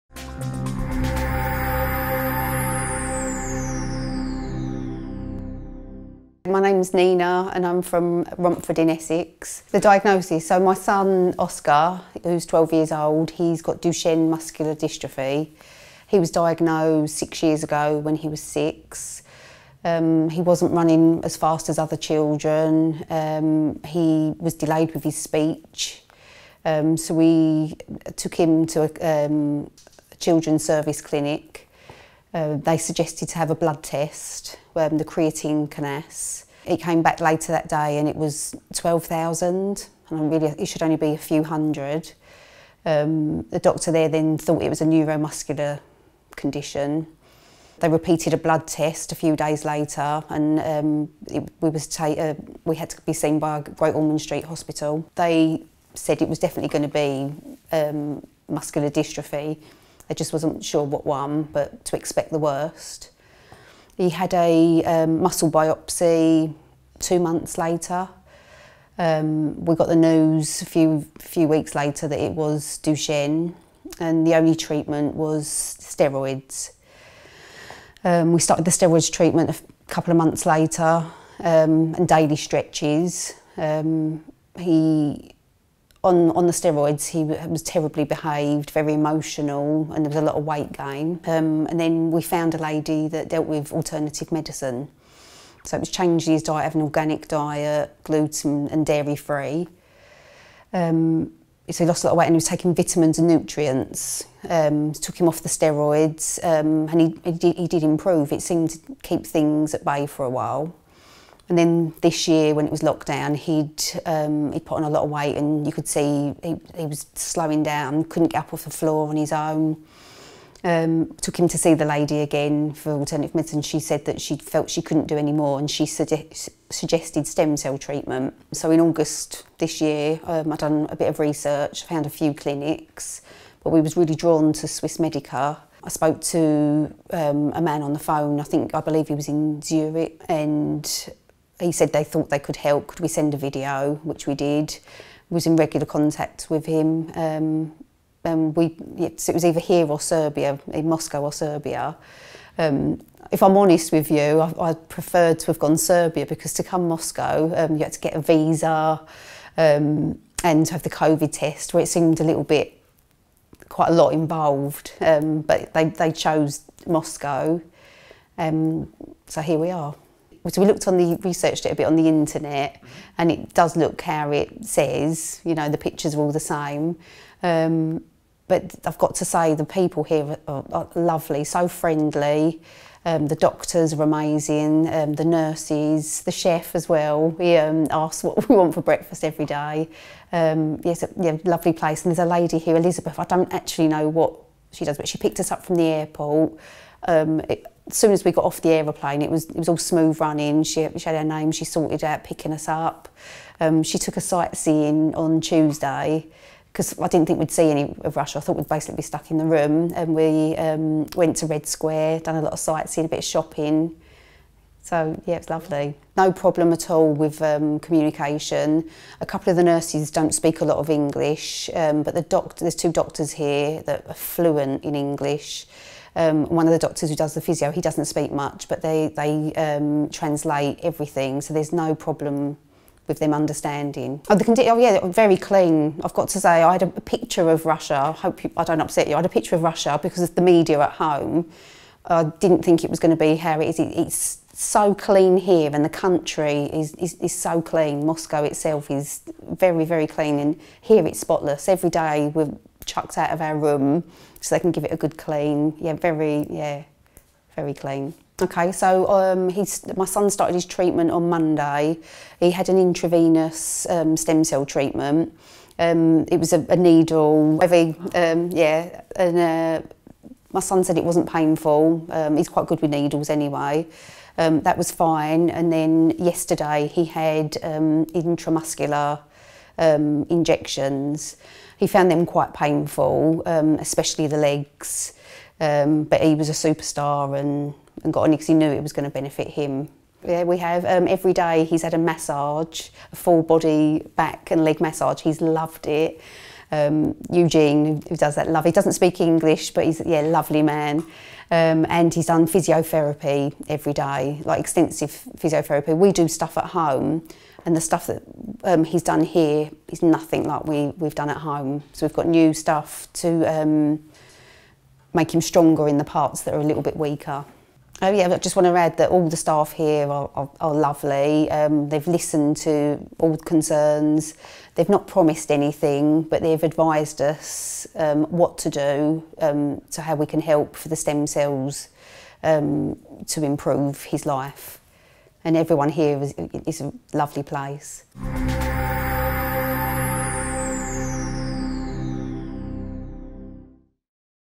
Stem Cell Therapy for Duchenne muscular dystrophy
Patient from the UK
Diagnosis: Duchenne muscular dystrophy
Duration: 6 years.
First visit to Swiss Medica.

Oscar was diagnosed with Duchenne muscular dystrophy when he was 6 years old.
The steroid treatment did not show any results either as alternative medicine. Nina was recommended to try stem cell treatment.
After the research among stem cell clinics, a choice was made to Swiss Medica.

A 5-day treatment program included:
Stem cell therapy:
1. Intramuscular infusion of allogeneic cultivated umbilical cord-derived multipotent mesenchymal stromal cells (100 mln)
2. Intravenous Infusion of allogeneic cultivated placenta-derived multipotent mesenchymal stromal cells.

To learn more about stem cell therapy and its expected results, visit our